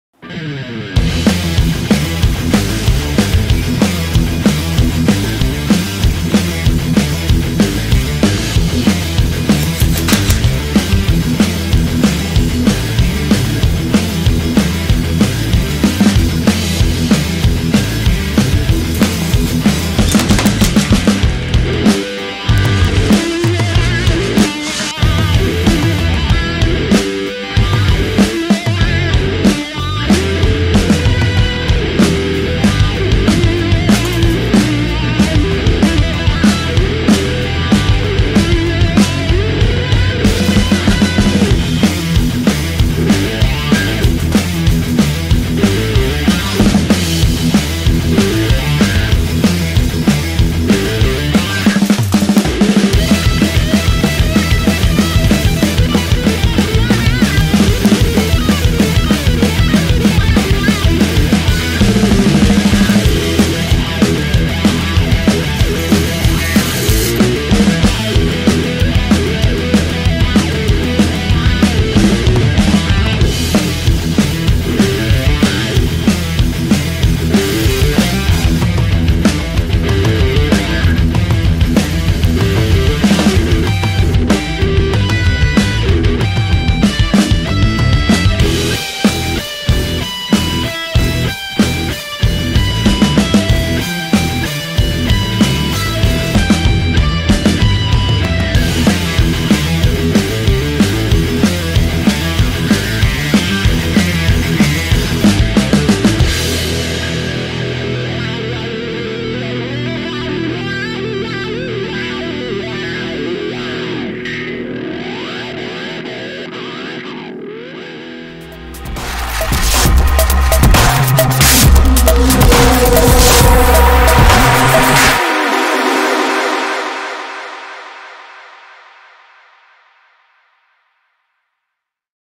Lego Robot Spider With A Turret On It's Back
Lego Technic RC spider with a gun turret that launches projectiles. The gun is as compact and light weight as possible. The leg stride has a very small vertical lift so the model can carry the extra weight of the gun without shaking itself to bits. Joining two previous models to make something new. This was a suggestion from the comments.